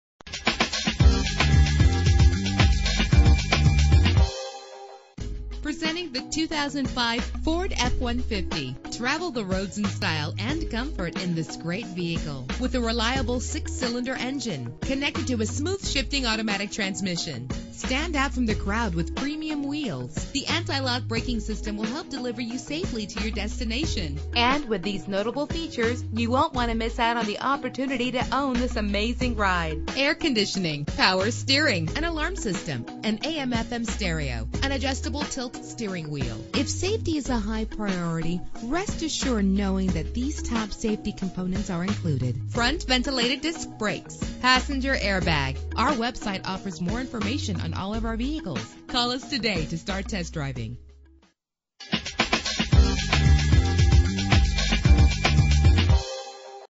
2005 Ford F-150 - Ft. Wayne IN
Year: 2005
Make: Ford
Model: F-150
Trim: XL
Engine: 4.2 liter 6 cylinder 12 valve
Transmission: 4-Speed Automatic
Color: Medium Graphite
Mileage: 20626
Address:
5000 Illinois Road
Ft. Wayne, IN 46804